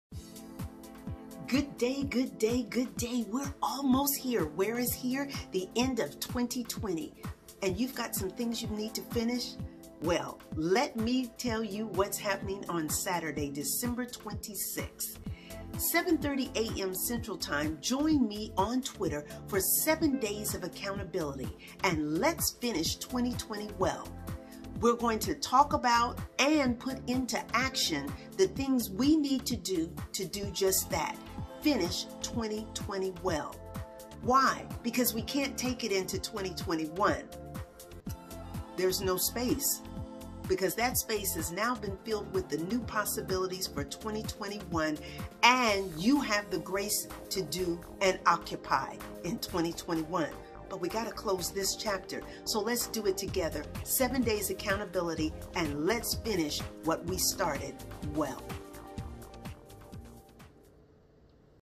7 Days of Accountability to Finish Well in 2020
What do you plan to leave in 2020...ALL the woulda, coulda, and didn'ts?

CONNECT & JOIN me on TWITTER, starting 12/26 @ 7:30am Central (GMT-6) for seven (7) days of self-care accountability to prepare for 2021 with clarity, renewed passion and a plan.

The cost is YOUR time & effort.